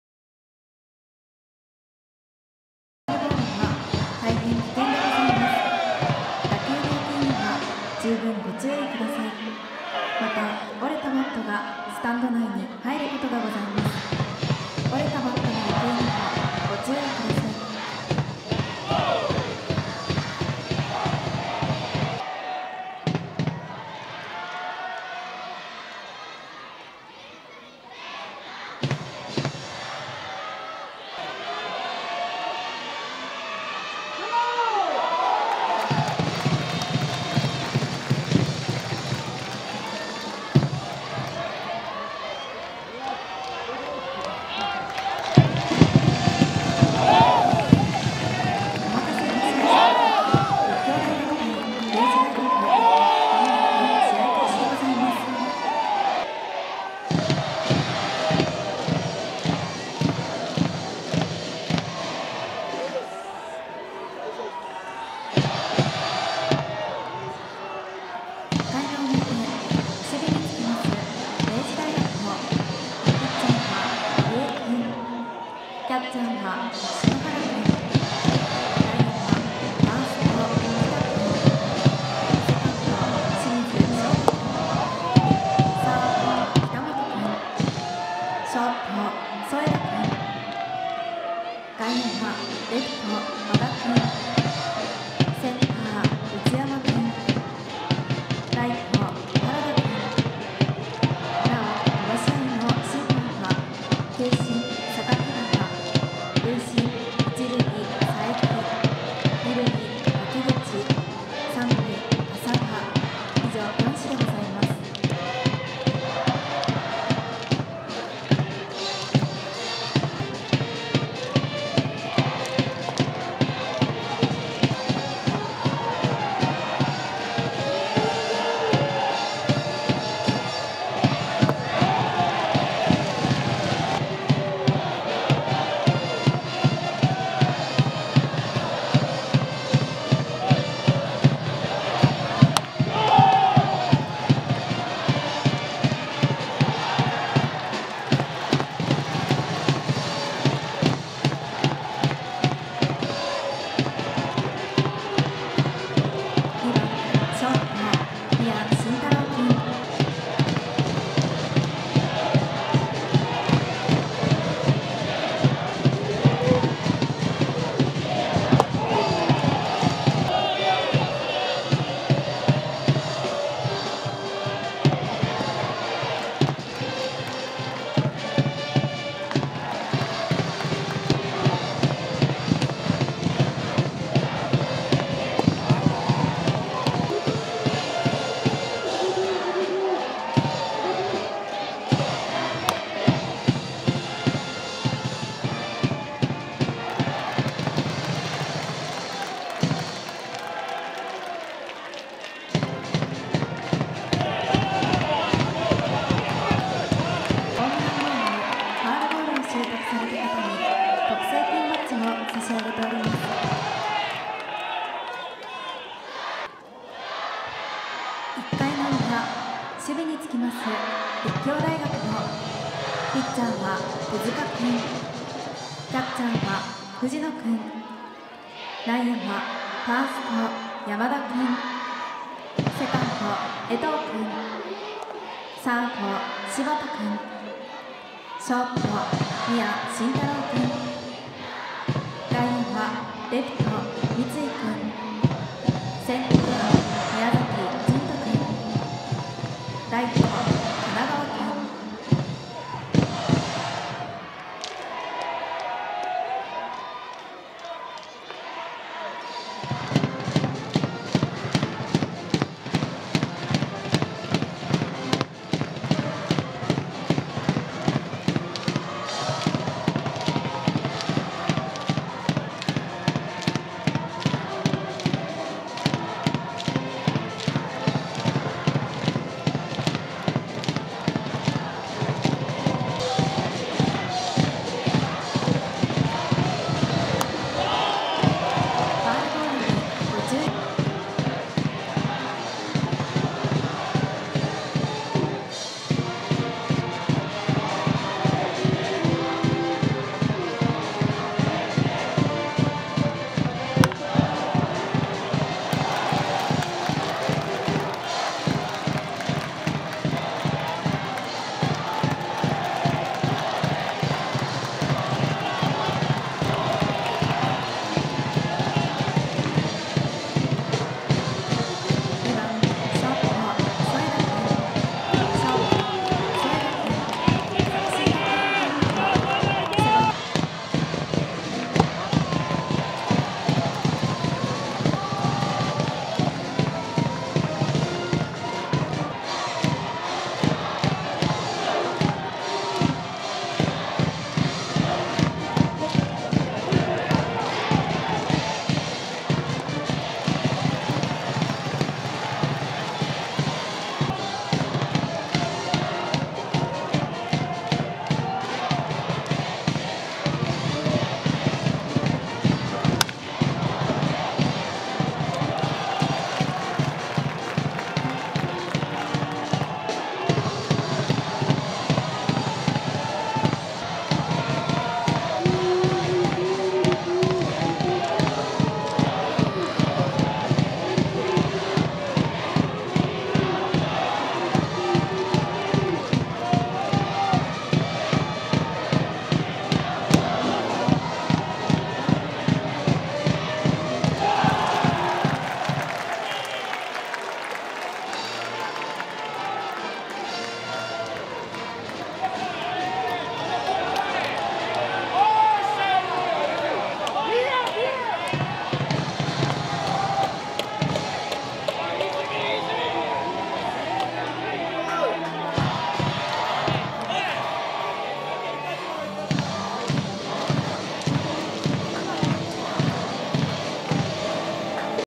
【ピックアップゲーム】東京六大学野球 2019年10月30日 立教VS明治（ラストゲーム） 1回 ①（2019年秋季リーグ戦）
２０１９年秋季リーグ戦、４年生のラストゲーム。
明治の先発は、入江大生投手（当時３年：横浜DeNAベイスターズ）、リリーフに伊勢大夢投手（当時４年：横浜DeNAベイスターズ）、森下暢仁投手（当時４年：広島カープ）が出場。森下投手は代打、レフト、投手と二刀流でラストゲームを飾る。立教の先発は４年生の手塚周投手、リリーフに中川颯投手（当時３年：オリックスバファローズ）、最後はエースの４年生／田中誠也投手が出場。このシーズンの両校の対決は、引分一つ入り、４回戦まで及んだ。
試合は１回終了時点で、０：０。